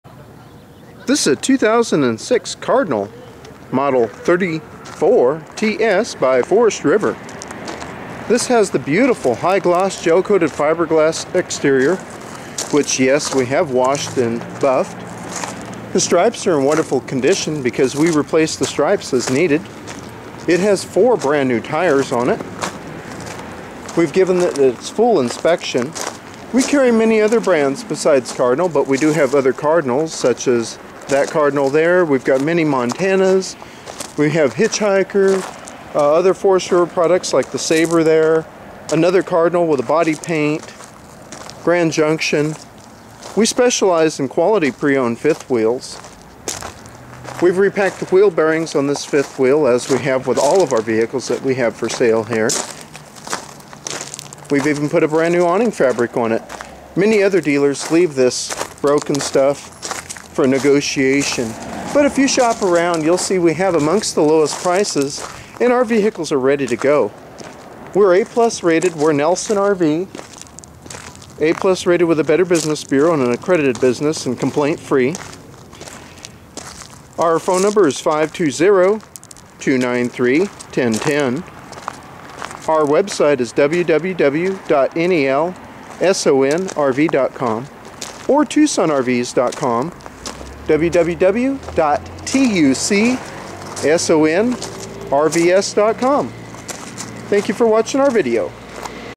2006 Forest River Cardinal 34TS for Sale
Empty Weight (UVW): 11,380 lbs. Hitch Weight: 2,410 lbs. 65 Gallon Fresh Water, 40 Gallon Grey, 40 Gallon Galley, 40 Gallon Black, Heated & Enclosed Holding Tanks, All Season Arctic Package, 50 amp power service (30 amp reducer included), Washer/dryer ready, King or queen bed (Currently set-up as king), Computer Desk, 4 New Tires, Alloy Wheels, Dual Ducted ACs (2 Separate AC Units!), Sofa/Hide-A-Bed, Electric Fireplace, Ceiling Fan, 12 volt power vent fan in bathroom (Above toilet), We can arrange Delivery & Set-up!